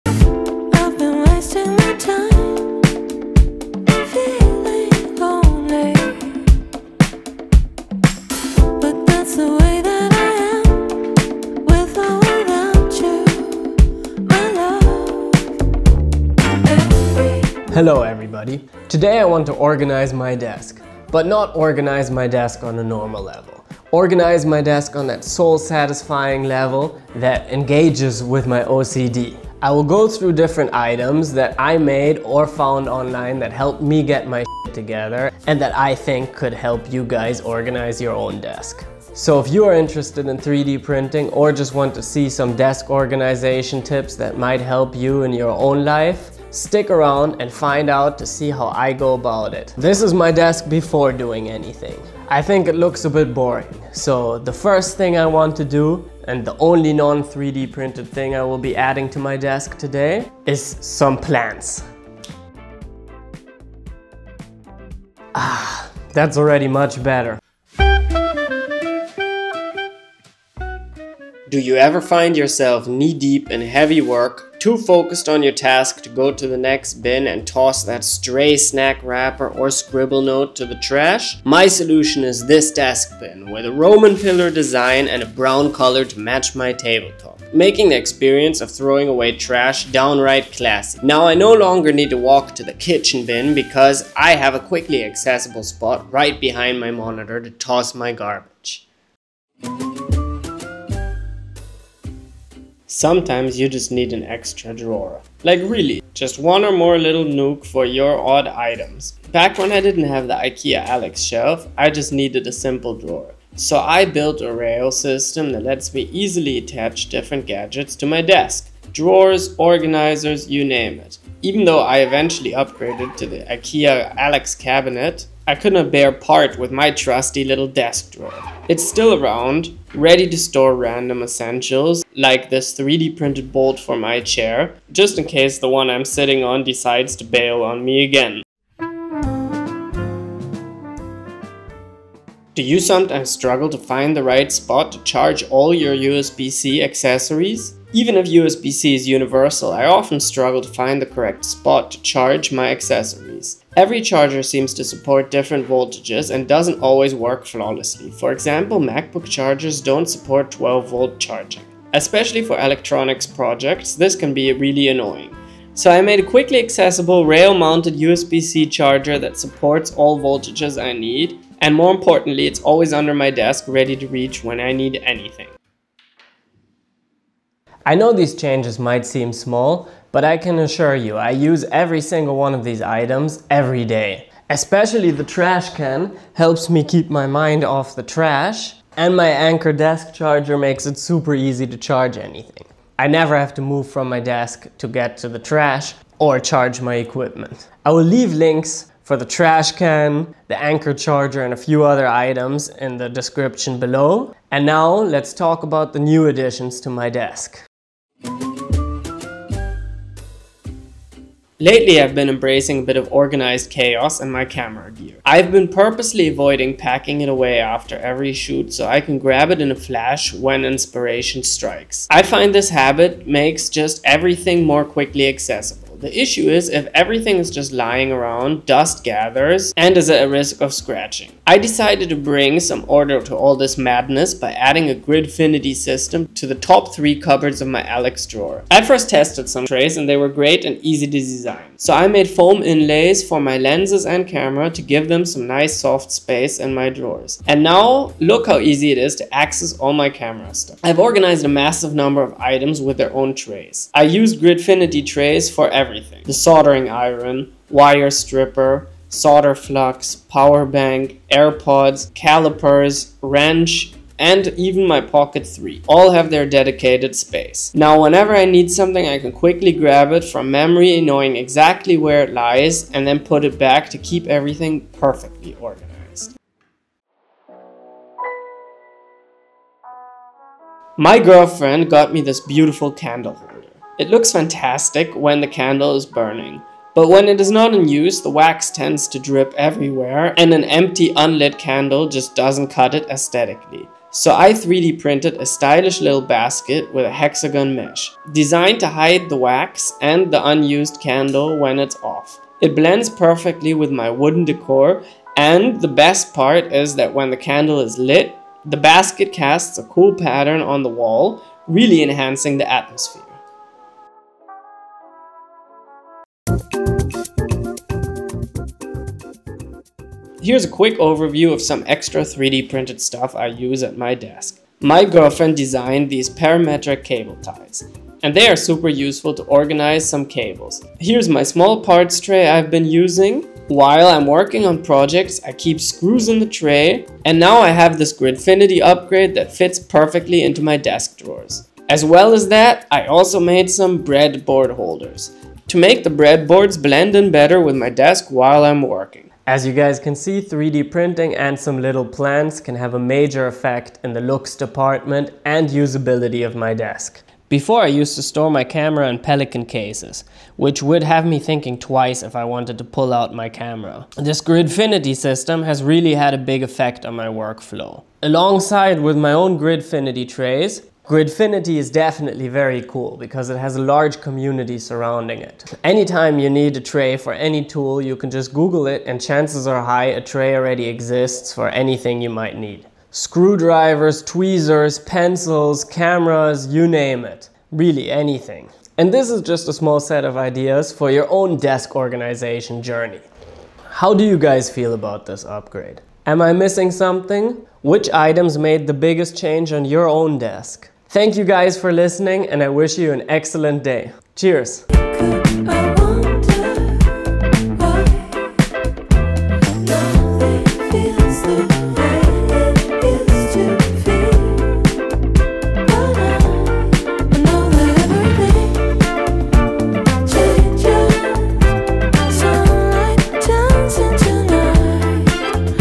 Pimp My Desk: 3D Printed Desk Organization
Upgrade your workspace with a few smart tweaks! In this video, I showcase different methods of desk organization using 3D Printing. From 3D foam inlays for lenses to Desk Mounted Chargers, see how small changes can boost both style and workflow. What upgrades have made a difference in your setup?